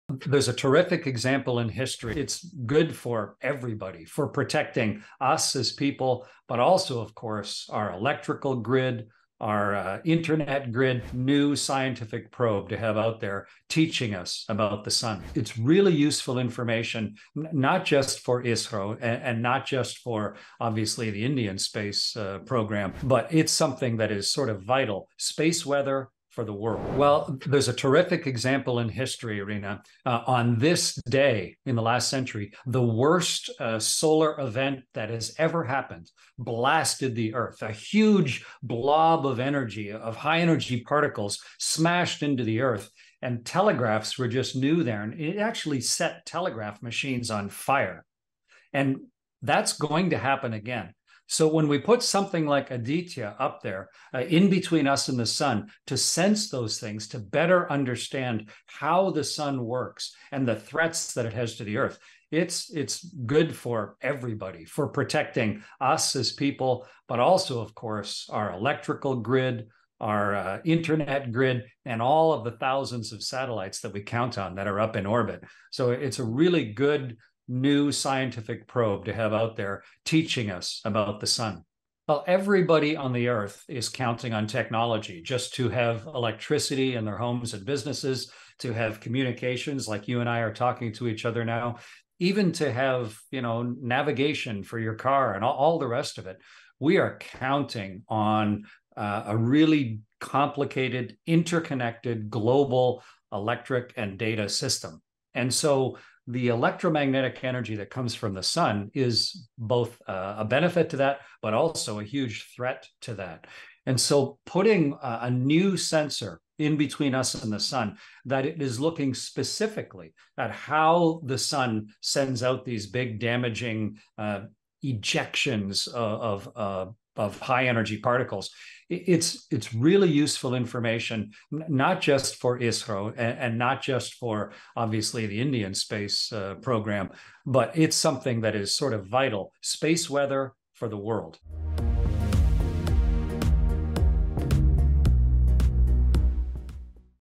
Aditya L1 Mission: Former ISS Commander Highlights Global Importance of ISRO's Sun Mission
Aditya L1 Mission: Renowned astronaut and author Chris Hadfield, known for his role as the former Commander of the International Space Station and his book "The Apollo Murders," highlighted the global dependence on technology and the critical role of the Aditya L-1 Mission by ISRO. He emphasized that humanity relies on an intricate worldwide electrical and data system, which is vulnerable to the Sun's electromagnetic energy, posing both benefits and threats. Hadfield praised the mission's significance, as it will place a vital sensor between Earth and the Sun to specifically monitor the Sun's potentially damaging high-energy particle ejections. This information, he noted, holds immense value not only for ISRO or India's space program but for the world at large.